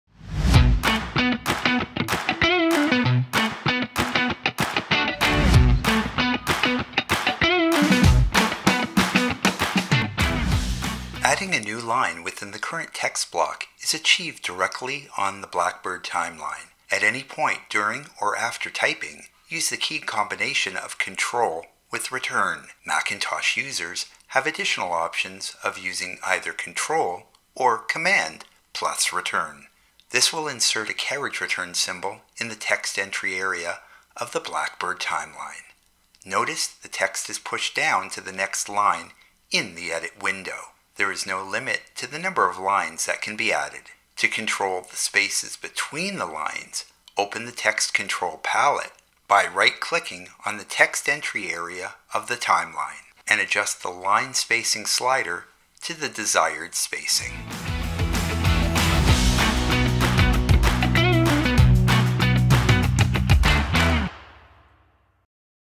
Line Spacing in Text
This video instructs you how to insert lines of text in the Timeline.